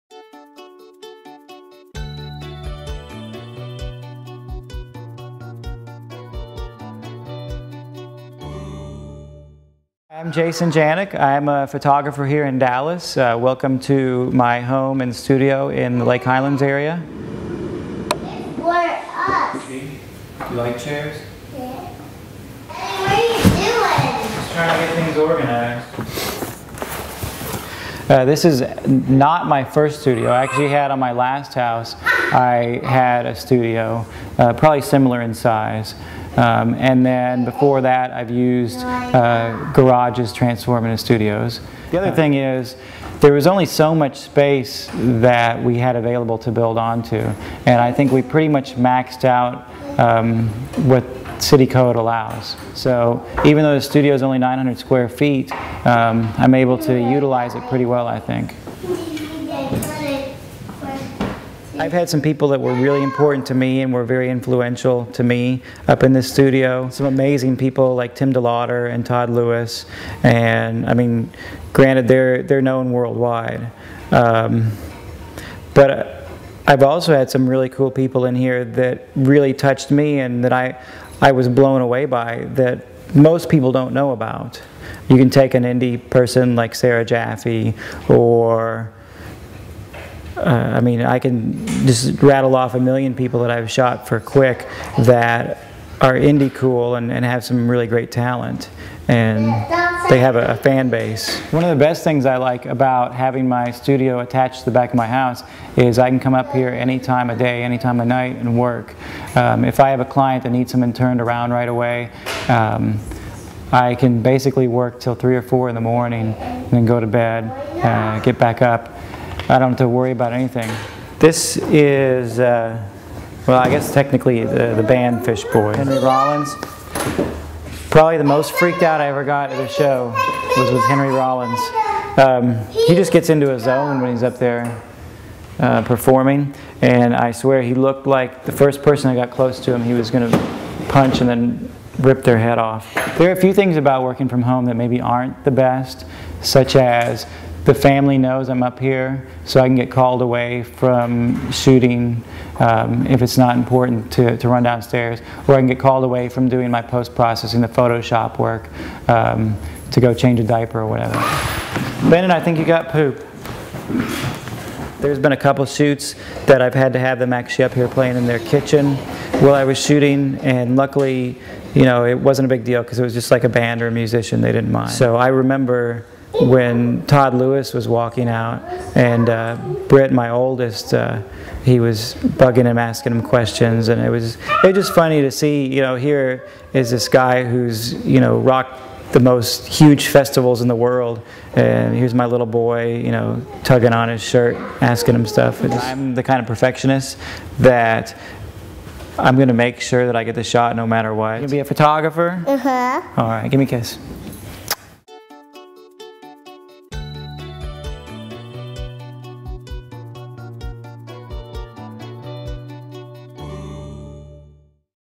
Artist Studio Tour With Jason Janik
KERA's Art&Seek take a peek inside the artist studio of Jason Janik.